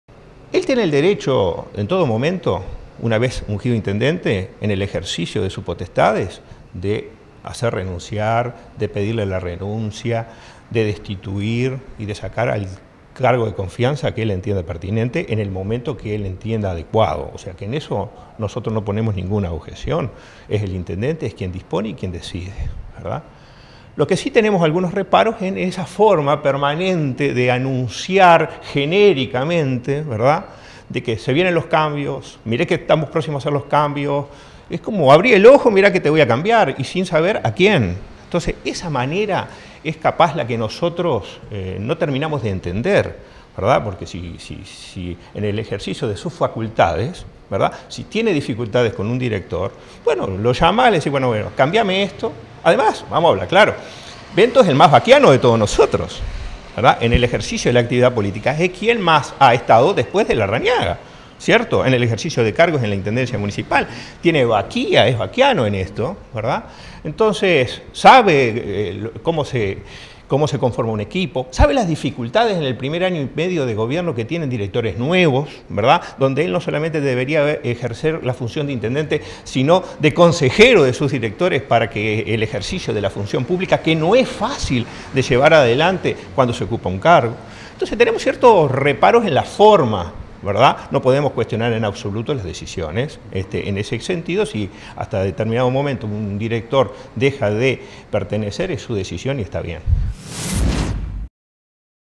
ARCIERI CAMBIOS EN INTENDENCIA.mpg
DANIEL ARCIERI / Lista 3 Partido Nacional
A TODO ESTO EL LIDER DE LA LISTA 3, DANIEL ARCIERI ,SECTOR AL QUE PERTENECE EL DR. TORTORELLA, CUESTIONO LA FORMA EN QUE EL INTENDENTE VIENE ANUNCIANDO CAMBIOS EN SU EQUIPO.
NO OBSTANTE ARCIERI DEJO EN CLARO QUE BENTOS TIENE LA ABSOLUTA POTESTAD DE HACER LOS CAMBIOS QUE SEAN NECESARIOS